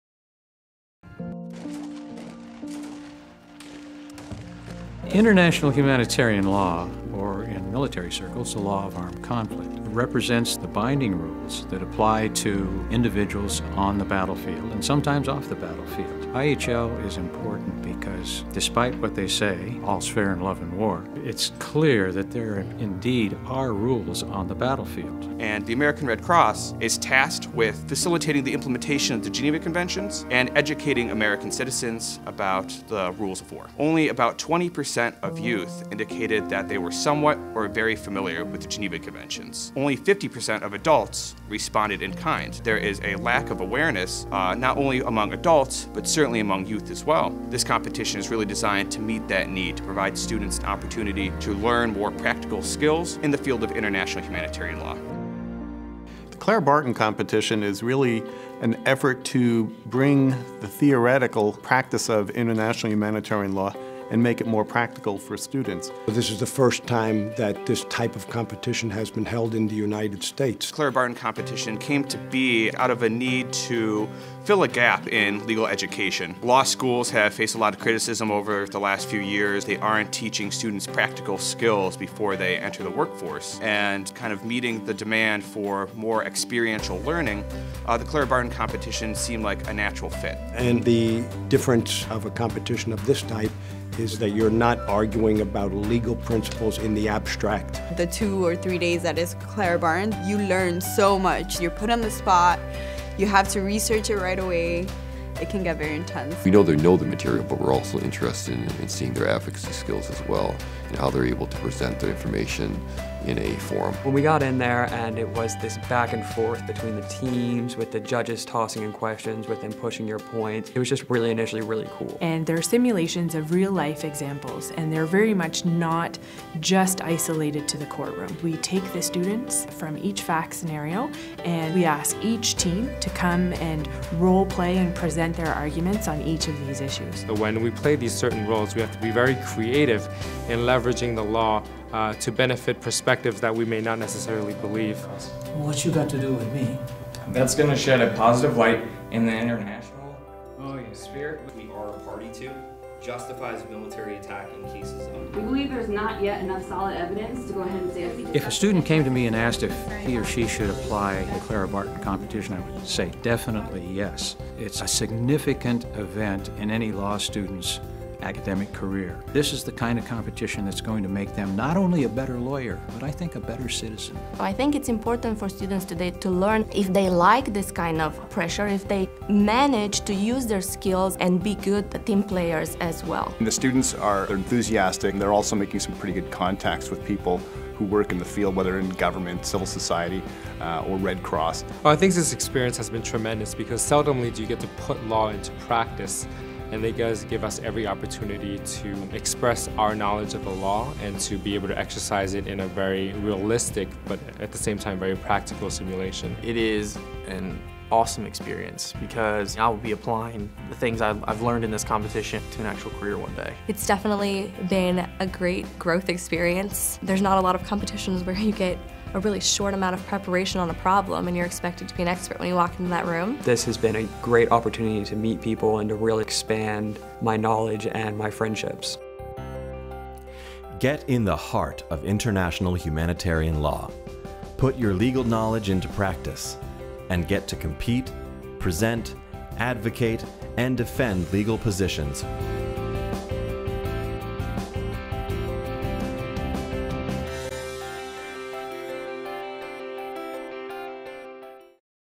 Clara Barton International Humanitarian Law Competition 2014
The Clara Barton International Humanitarian Law Competition is a simulation-based, experiential legal competition designed to expose rising professionals to the practice of IHL and to contemporary challenges facing IHL practitioners during armed conflict.

During the Competition, participants engage in a variety of practical role playing exercises which require them to assume various professional roles and accomplish a wide range of tasks reflective of those per¬formed by practitioners in the field. The Clara Barton Competition breaks from the format of typical moot court competitions that pres¬ent static, two-sided debate through the recitation of legal briefs, and instead utilizes a dynamic case study based approach to take the law out of the books and expose students to the real world practice of law.